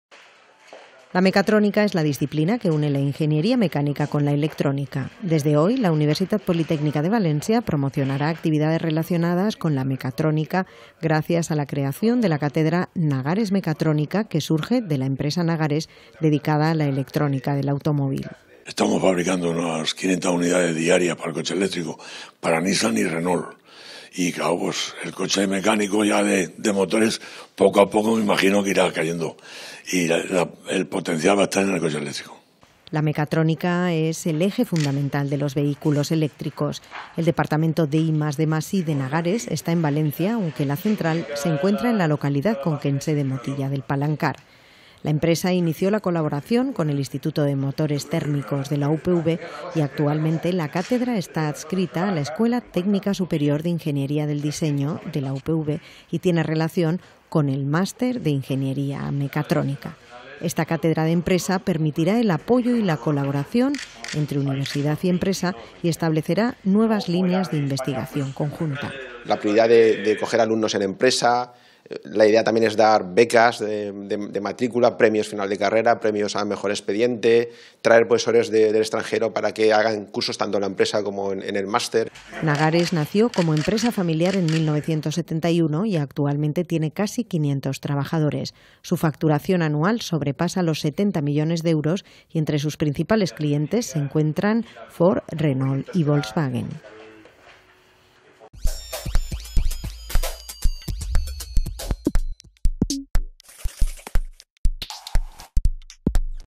Noticias @UPVTV: Cátedra Nagares [08-09-2015]
La mecatrónica es la disciplina que una la ingeniería mecánica con la electrónica. Se utiliza… Desde hoy la Universitat Politècnica de Valencia promocionará más actividades relacionadas con la mecatrónica gracias a la formalización de la cátedra Nagares…

Desde la UPV se promocionan las cátedras de empresa como instrumento de colaboración de la universidad con la sociedad. Esta cátedra está adscrita a la Escuela Técnica Superior de Ingeniería del Diseño (ETSID) y prevé la organización de diferentes actividades orientadas a dar a concer la mecatrónica, así como la promoción de prácticas en empresa y abrir nuevas líneas de investigación conjunta

Eduardo García Breijo, profesor del Departamento de Ingeniería Electrónica y miembro del Centro de Reconocimiento Molecular y Desarrollo Tecnológico (IDM).

Durante el acto, el presidente de Nagares, SA, Herminio Navalón, ha declarado que “el valor adicional de la cátedra es unir actividades de formación, investigación e innovación con el objetivo de ganar en eficiencia en la colaboración entre las dos instituciones y facilitar una mayor transferencia de conocimiento hacia el mundo de la industria”.

Nagares es una empresa familiar fundada en 1971. Está en Motilla del Palancar, en Cuenca y da trabajo a casi 500 personas. Su facturación anual sobrepasa los 70 millones de euros y entre sus principales clientes se encuentran, entre otros, Ford, Renault, Nissan, Volkswagen y Peugeot.